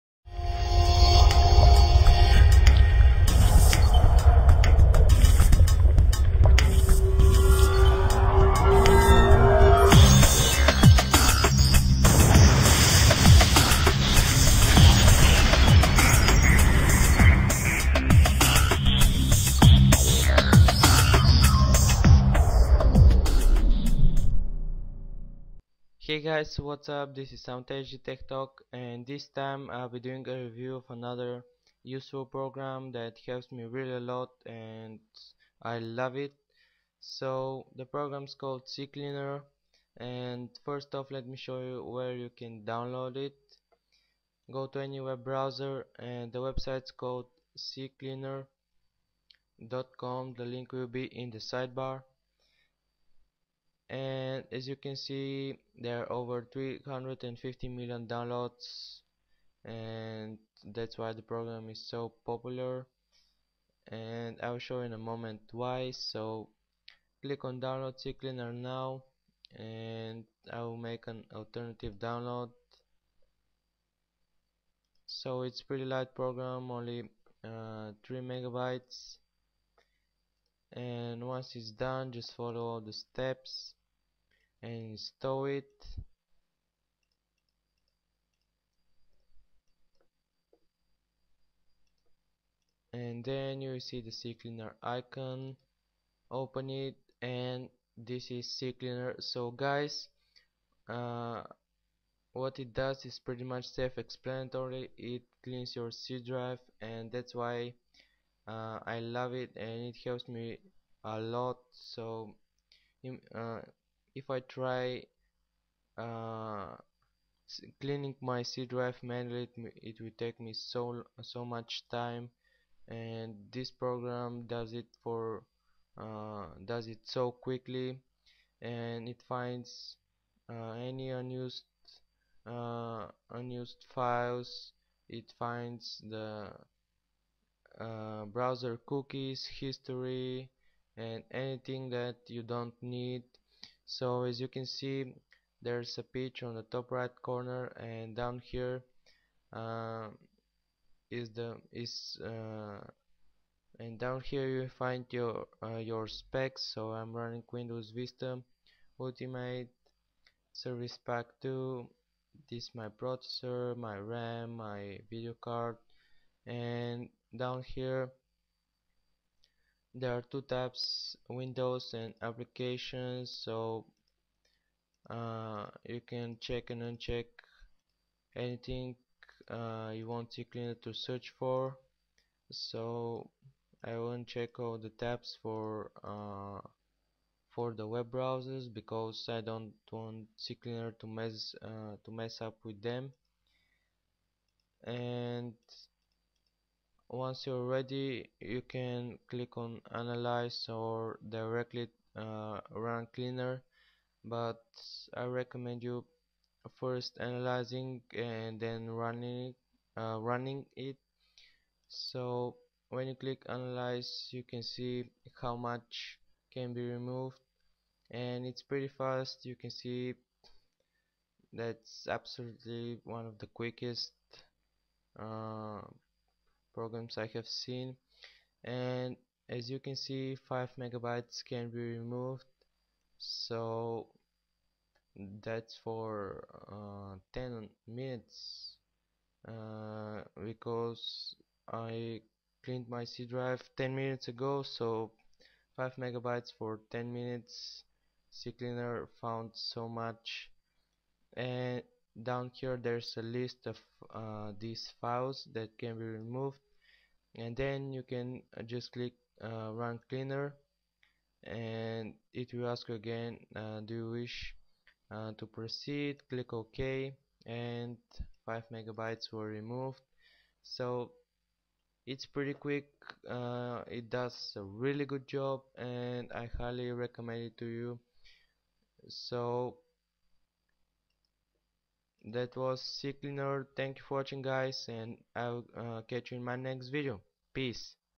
HD Review : Ccleaner
This is another helpful tutorial in which I will be showing you guys Ccleaner!It's a small and neat program that does such a good work!